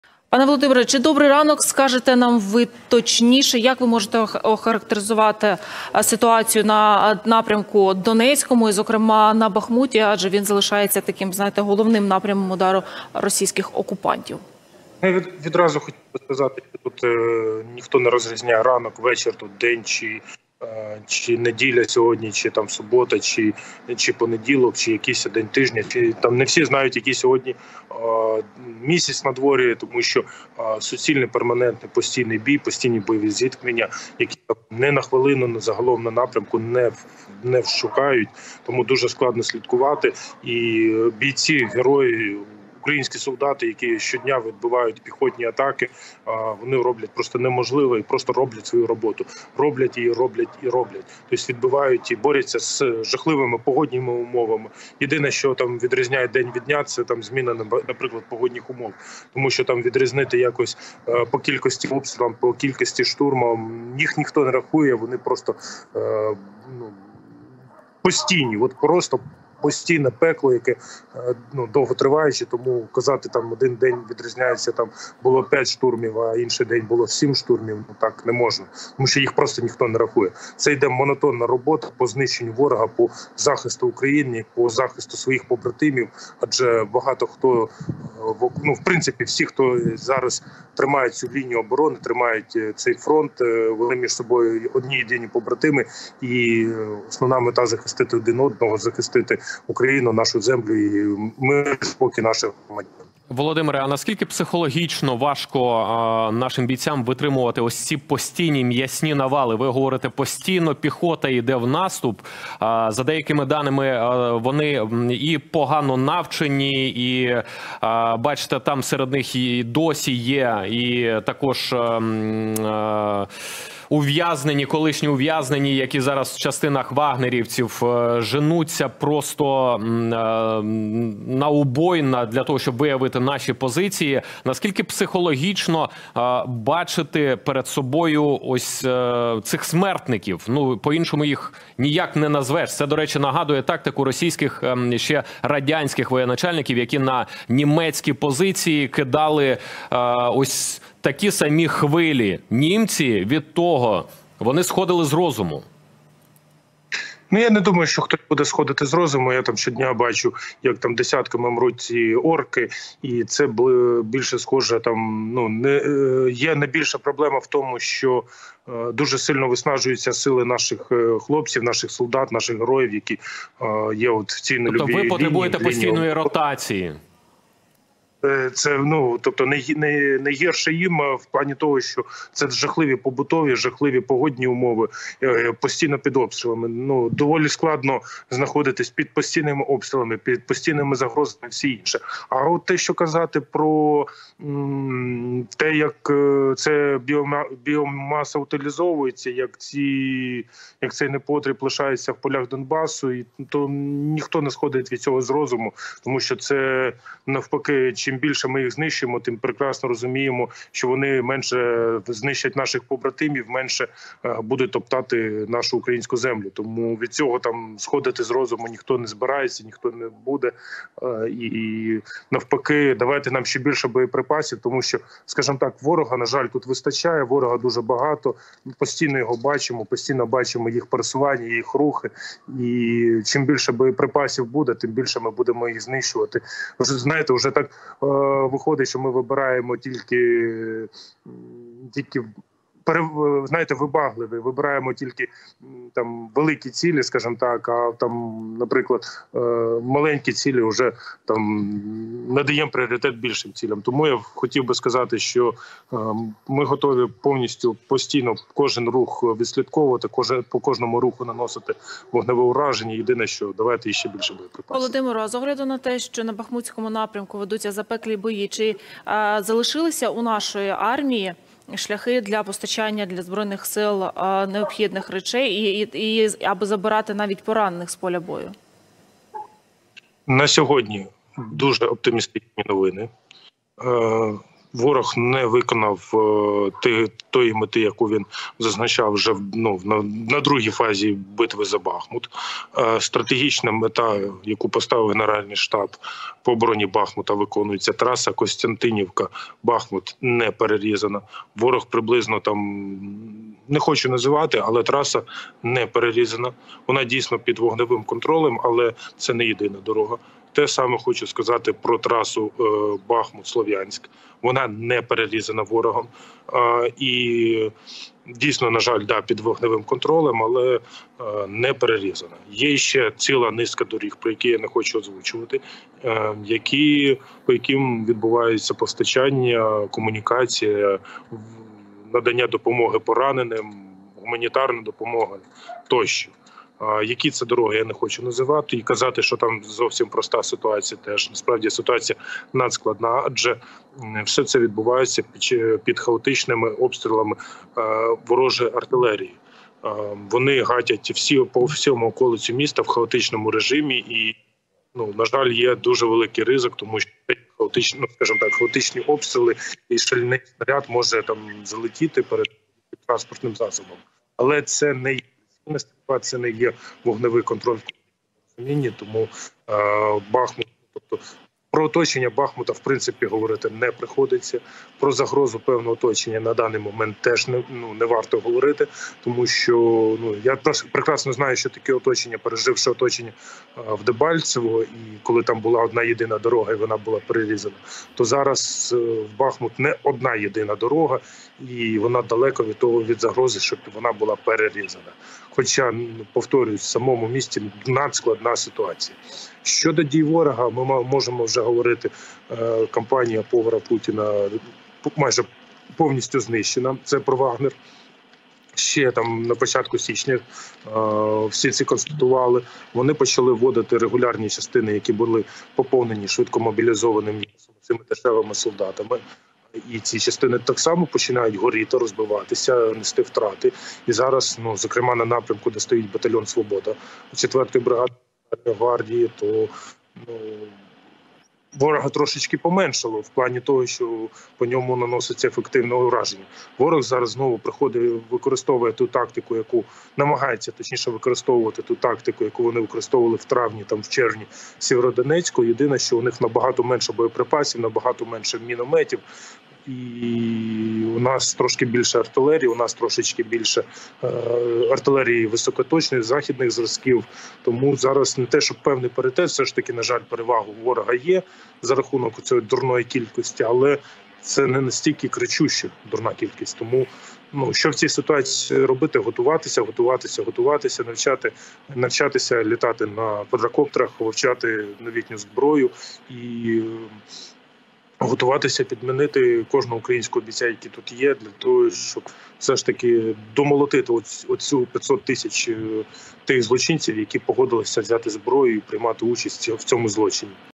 Бахмут: про оточення міста чи навіть загрозу нині не йдеться, — Володимир Назаренко / Легіон Свободи
Бахмутський напрямок: фронтові новини від Володимира Назаренка, лейтенанта «Легіону Свободи», заступника командира 3-го батальйону оперативного призначення «Свобода» у складі Бригади швидкого реагування Національної гвардії України "Рубіж".
Етер телеканалу "Рада" 11 лютого 2023 року